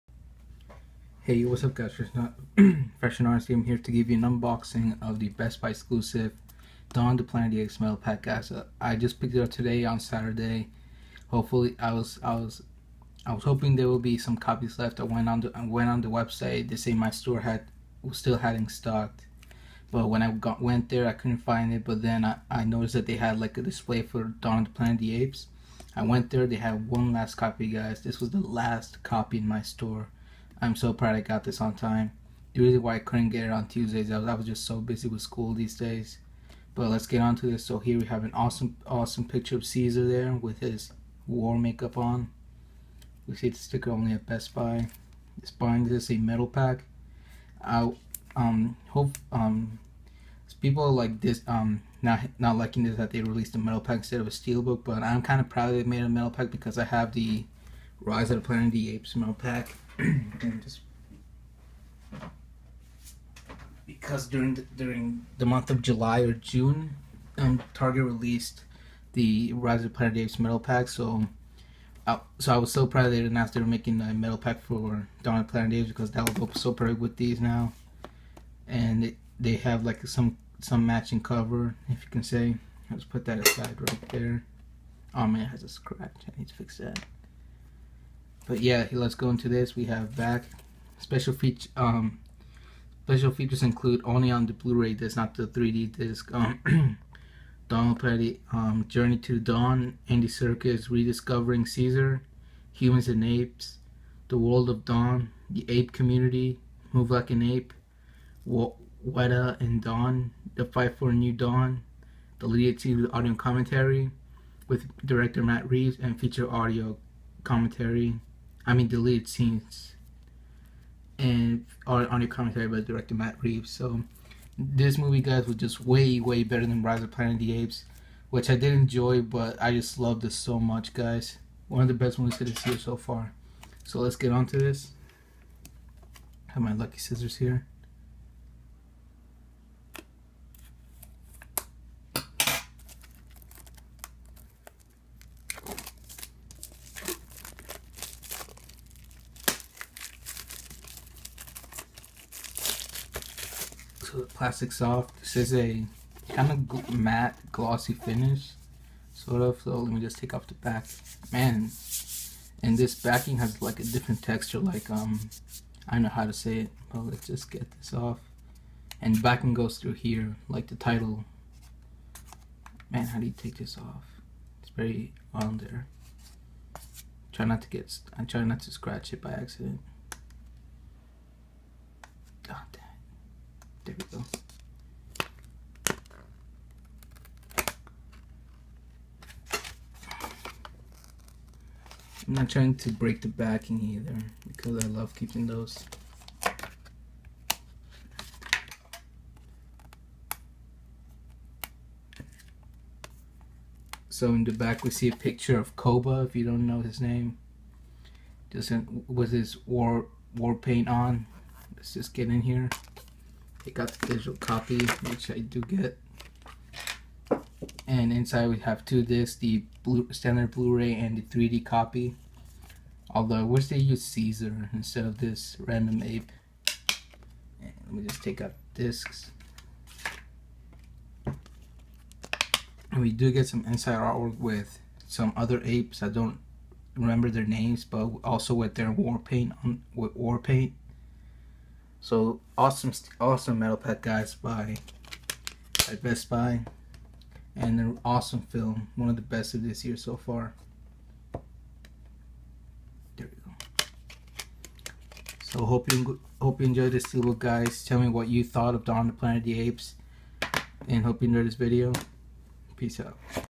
Dawn of the Planet Apes Metalpak Unboxing
I got a chance to get the final copy of the Dawn of the Planet of the Apes Metalpak at my local Best Buy. I got so lucky!